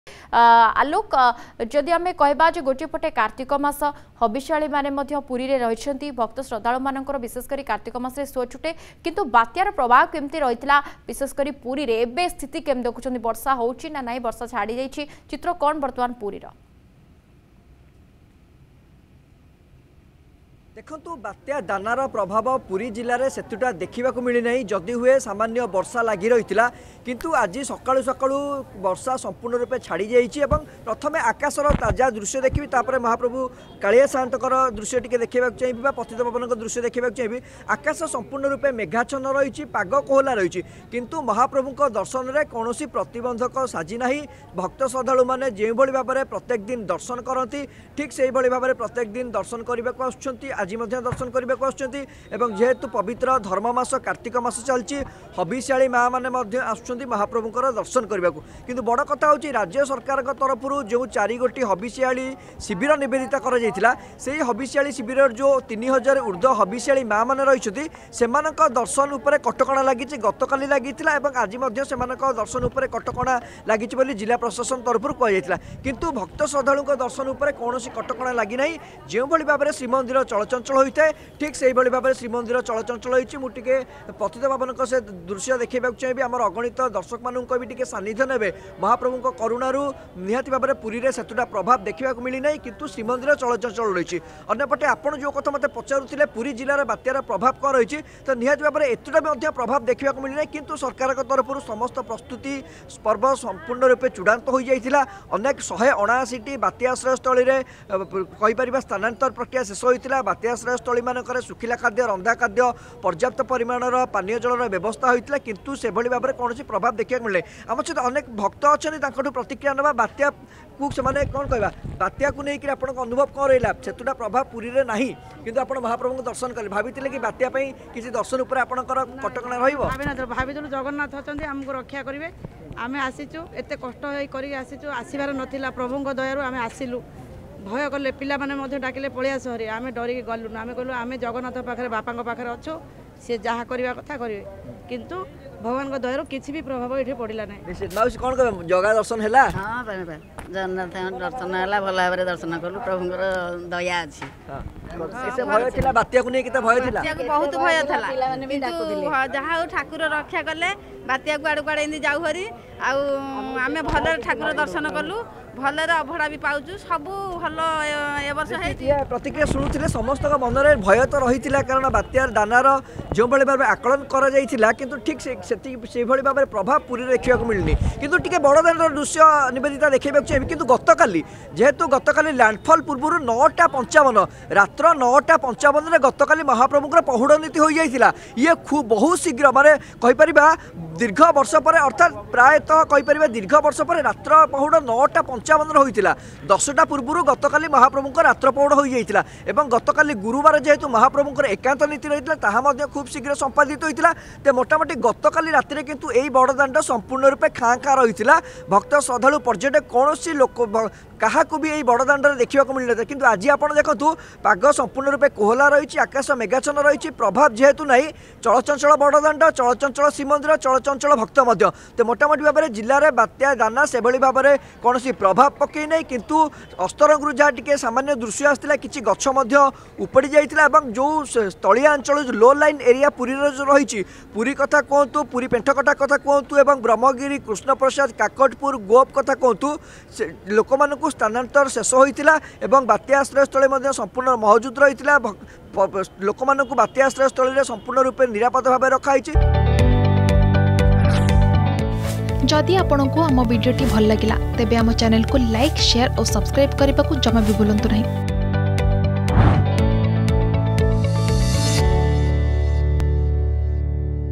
ବାତ୍ୟା 'ଦାନା'ର ପୁରୀରେ ପ୍ରଭାବ କିଛି ପଡିନାହିଁ | Cyclone Dana Update
ବାତ୍ୟା 'ଦାନା'ର ପୁରୀରେ ପ୍ରଭାବ କିଛି ପଡିନାହିଁ । ଶ୍ରୀମନ୍ଦିରରେ ମହାପ୍ରଭୁଙ୍କ ଦର୍ଶନ କରିଛନ୍ତି ଭକ୍ତ ଶ୍ରଦ୍ଧାଳୁ ।